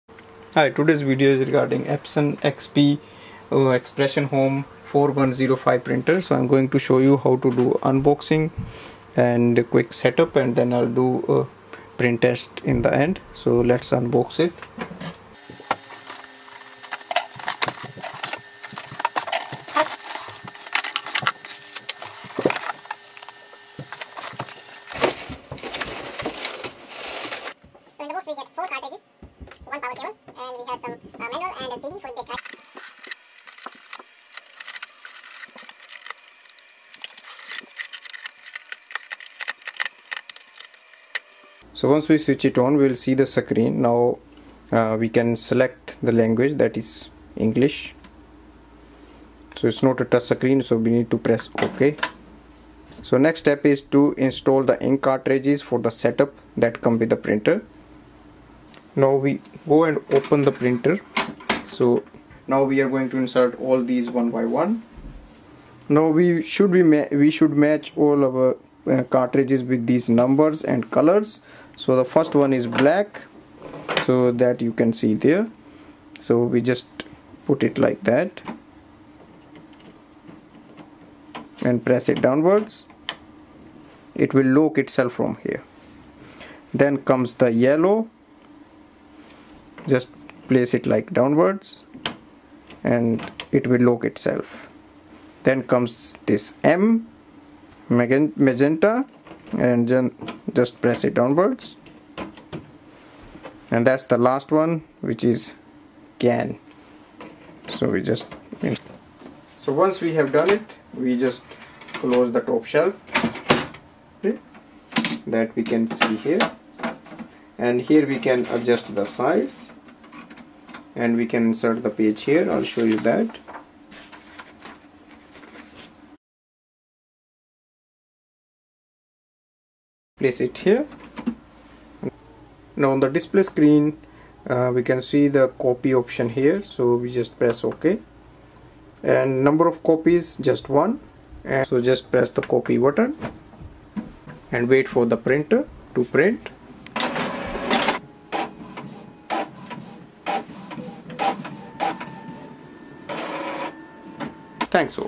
Epson XP-4105 SetUp (Initial SetUp) Install Setup Ink & Copy Test 🖨.
This video reviews the step-by-step method to unbox and set up your new Epson XP-4105 all-in-one printer. You learn initial setup and method to install ink cartridges and loading paper tray with a copy test in the end.